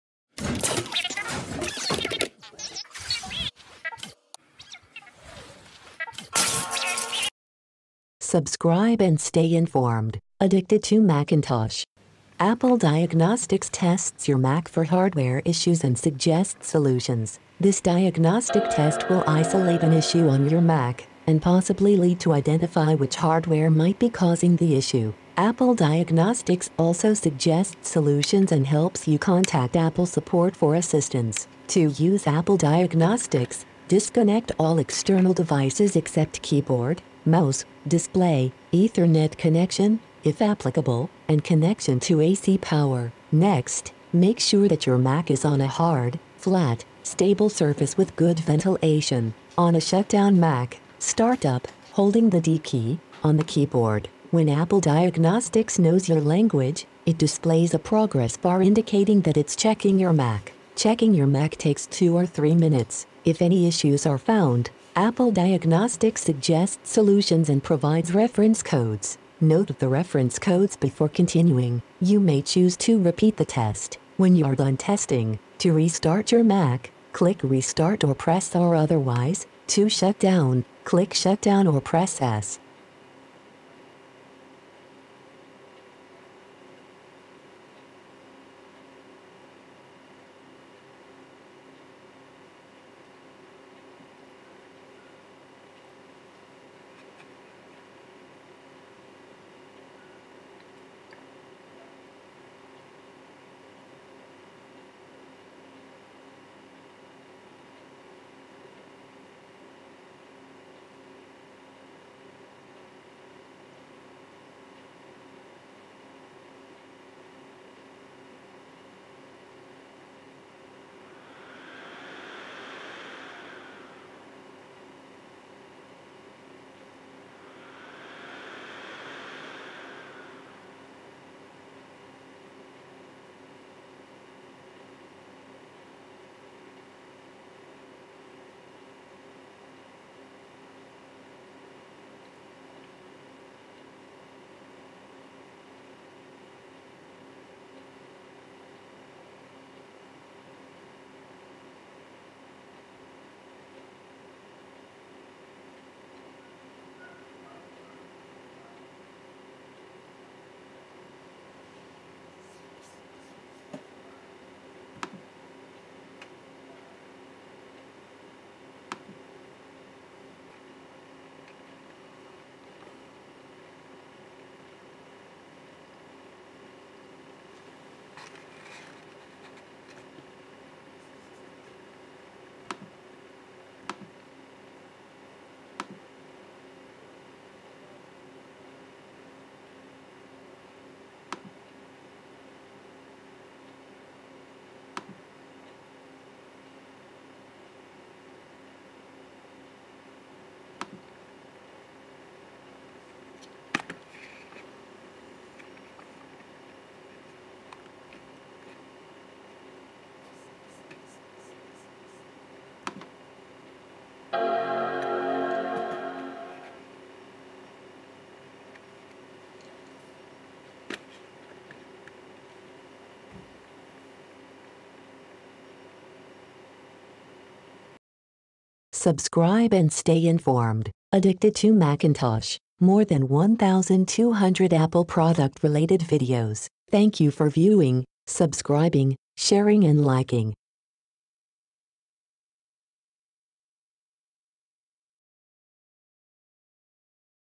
How to use Apple Diagnostics on your Mac
Are you suspecting that your mac is malfunctioning? Here is how to do a quick hardware diagnostic test.